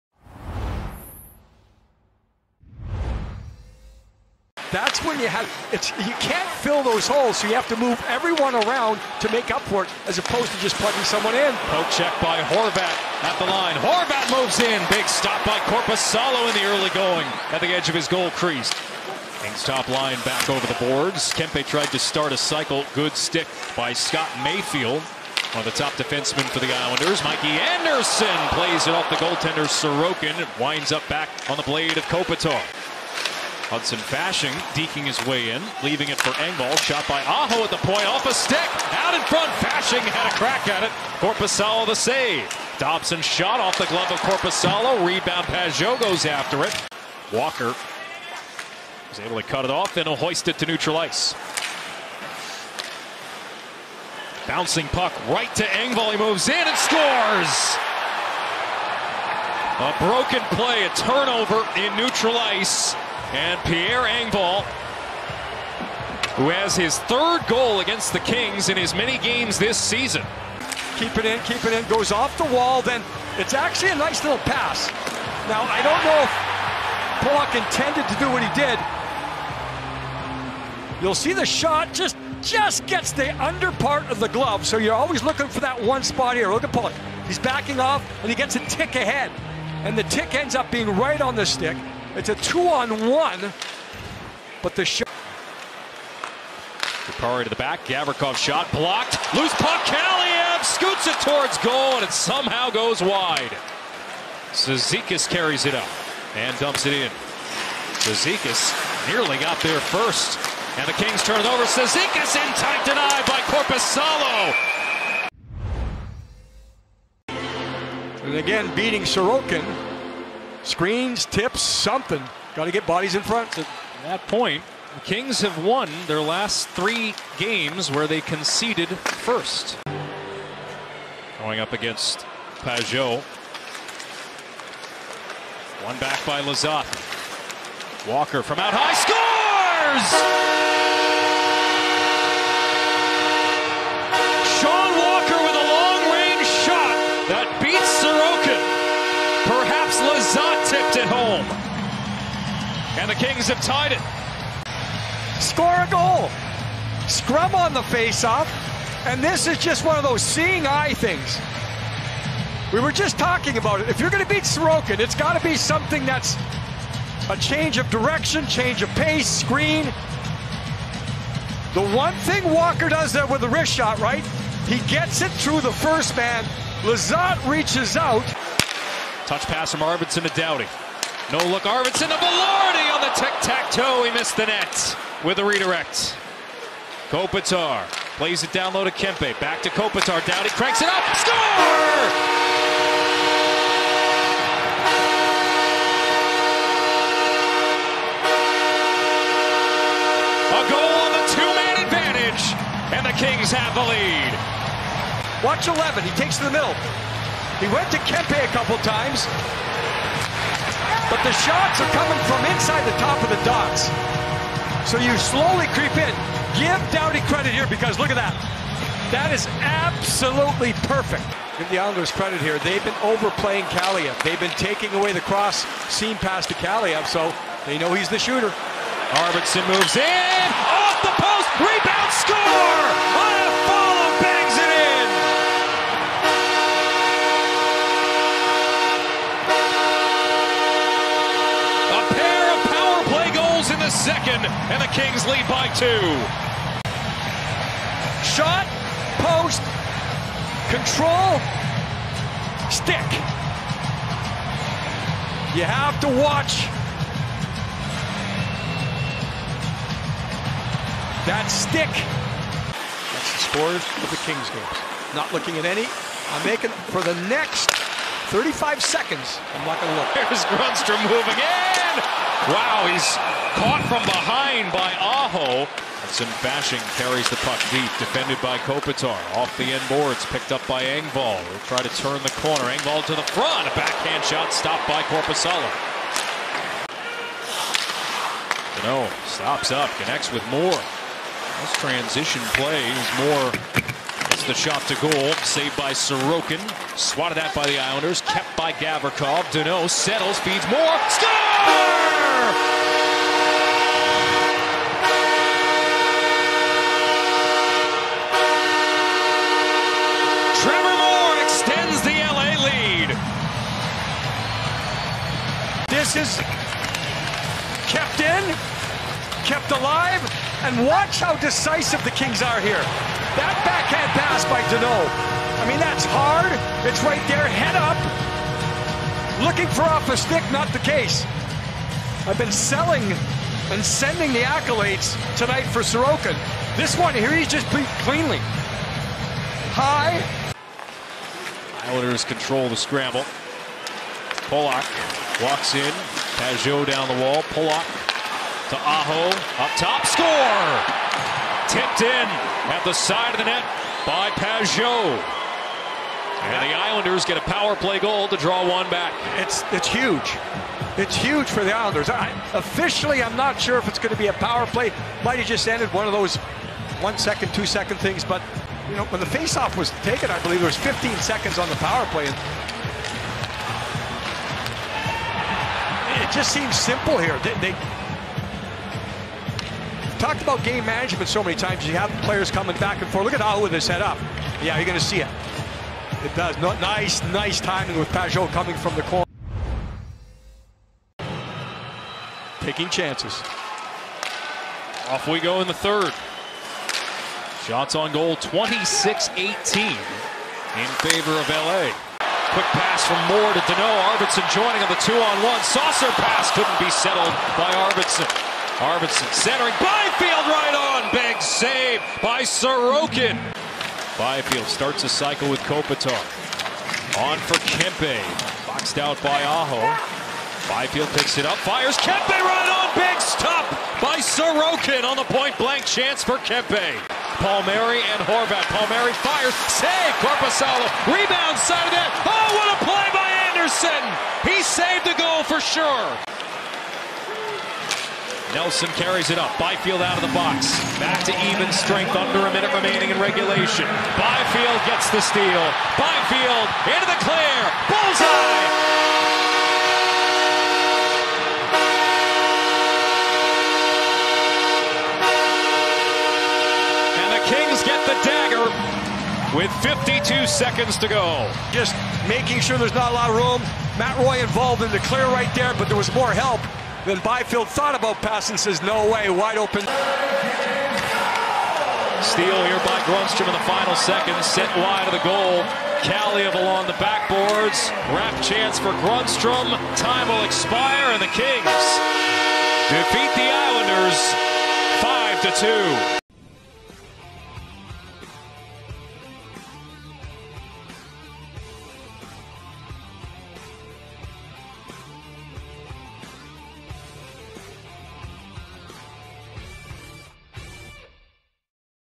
NHL Highlights | Islanders vs. Kings - March 14, 2023
Five different Kings scored, including Blake Lizotte's opener and Alex Iafallo's eventual game-winner, and Los Angeles defeated the New York Islanders 5-2.